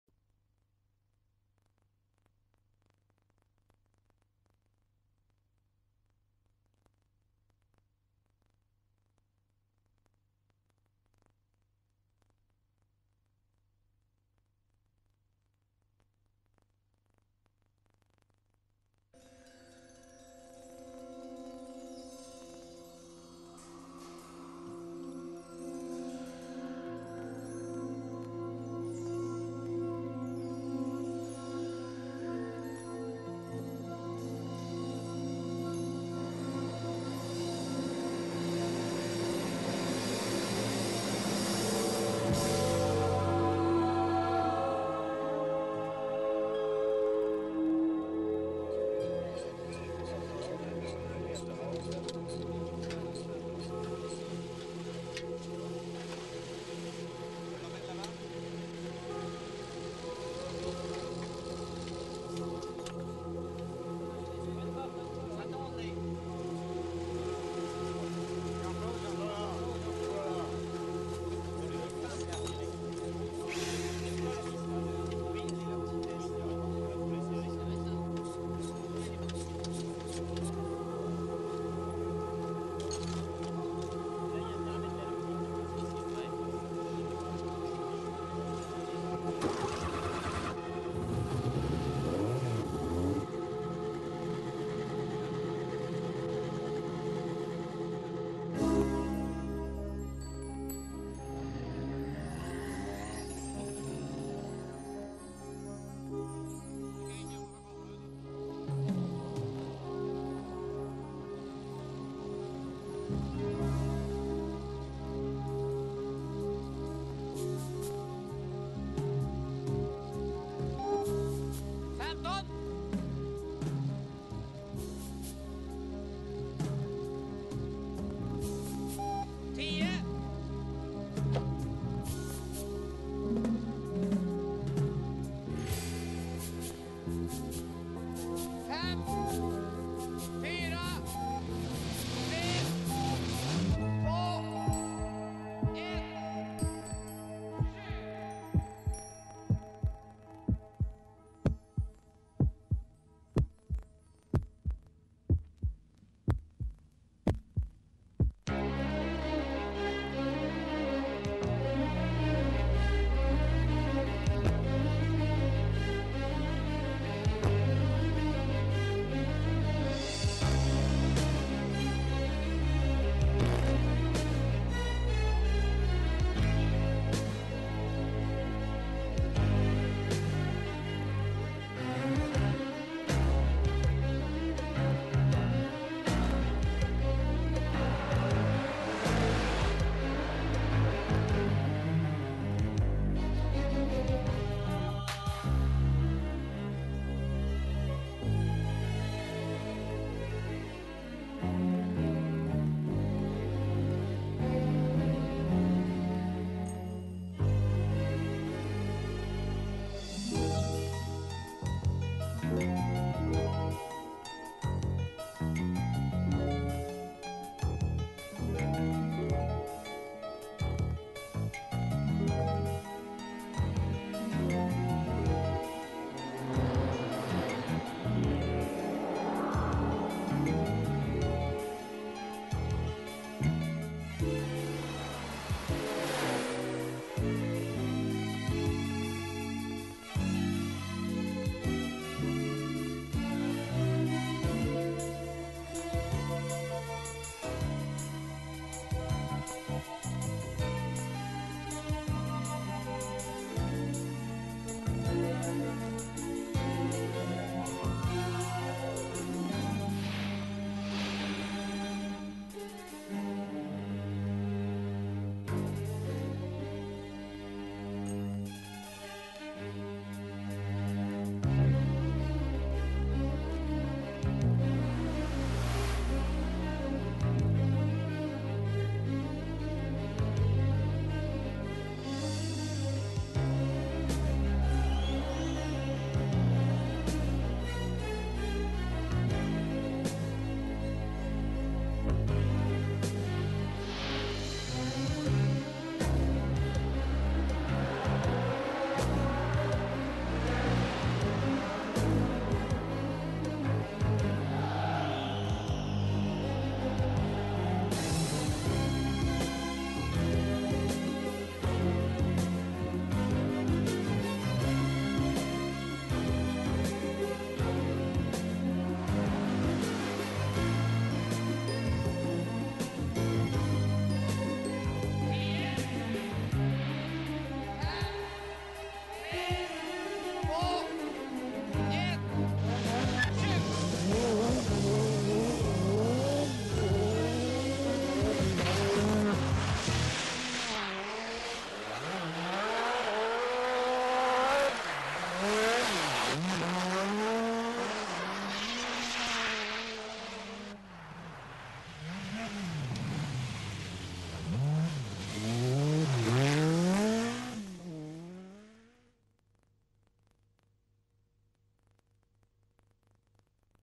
Group B - Timo Salonen Peugeot 205 T16
1985 World Rally Champion in action on snow